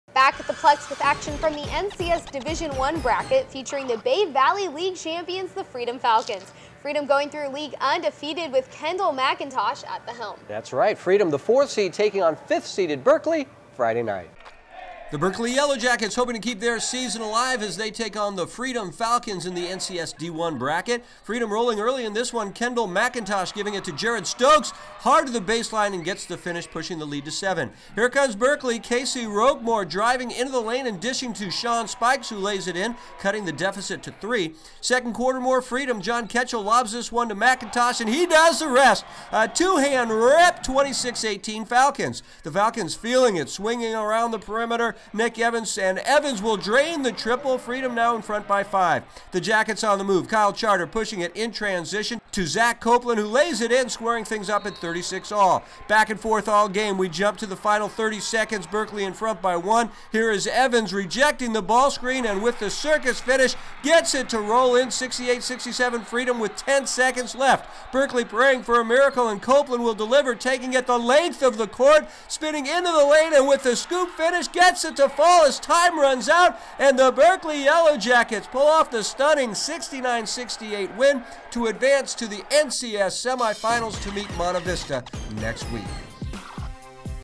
Berkeley at Freedom Boys Hoops
SO THE REAL QUESTION IS WHO CAN DANCE BETTER? WELL, BOTH THE YELLOWJACKETS AND THE FALCONS CAN DANCE BETTER THAN ME SO LET'S GET TO THE ACTION..
FREDOM ROLLING EARLY IN THIS ONE...KENDALL MCINTOSH GIVING IT TO JARED STOKES WHO TAKES IT HARD BASELINE AND GETS THE FINISH PUSHING THE LEAD TO SEVEN
BUT HERE COMES BERKELEY. CASEY ROQUEMORE...DRIVING INTO THE LANE AND DISHING TO SEAN SPIKES WHO LAYS IT IN CUTTING THE DEFICIT DOWN TO JUST THREE
SECOND QUARTER MORE FREEDOM...JOHN KETCHEL LOBS THIS ONE UP TO MCINTOSH AND HE WILL DO THE REST WITH THE TWO HAND SLAM 26-18 FALCONS
THE FALCONS FEELING IT SWINGING IT AROUND THE PERIMETER TO NICK EVANS AND EVANS WILL DRAIN THE TRIPLE FREEDOM NOW INFRONT BY FIVE
THE YELLOWJACKETS ON THE MOVE...KYLE CHARTER PUSHING IT IN TRANSISTION FINDS ZACH COPLEAND WHO LAYS IT IN... SQUARING THINGS UP AT 36
IT WAS BACK AND FORTH ALL GAME...WE WILL JUMP ALL THE WAY TO THE FINAL 30 SECONDS OF THE GAME BERKELEY INFRONT BY ONE...BUT HERE IS EVANS REJECTING THE BALL SCREEN AND WITH THE CIRCUS FINISH GETS IT TO ROLL IN...68-67 FREEDOM WITH 10.2 LEFT ON THE CLOCK
BEREKELY PRAYING FOR A MIRACLE AND COPELAND WILL DELIVER...TAKING IT THE LENGTH OF THE COURT..SPINNIGN INTO THE LANE ANE WITH THE SCOOP FINISH GETS IT TO FALL AS TIME WILL EXPIRE AND THE YELLOWJACKETS PULL OFF THE STUNNING 69-68 WIN TO ADVANCE TO THE NCS SEMIS